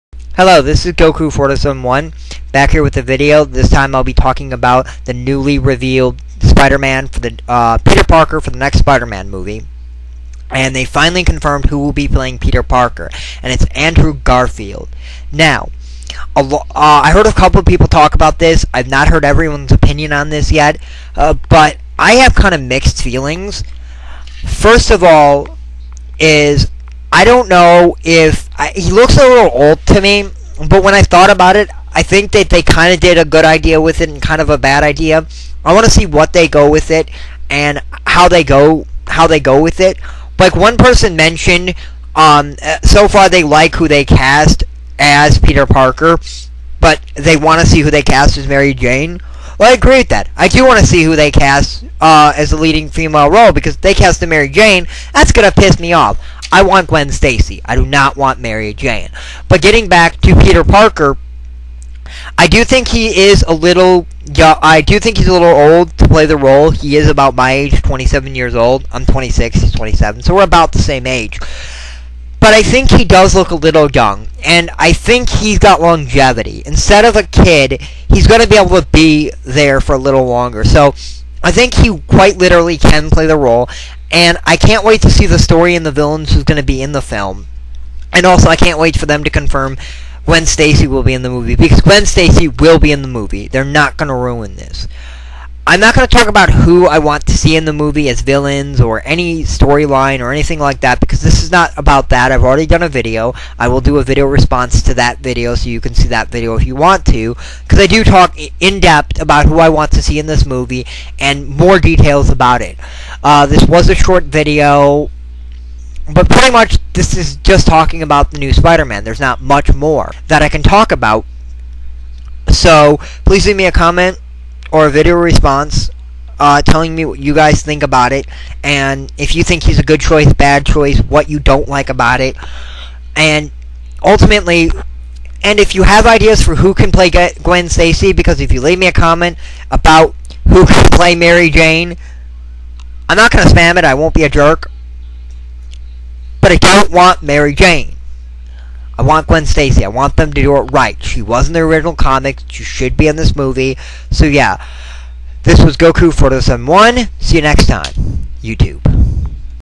My Thoughs on the New Peter Parker aka Spider-Man
This is my thoughs on who Sony has get to play Peter Parker aka Spider-Man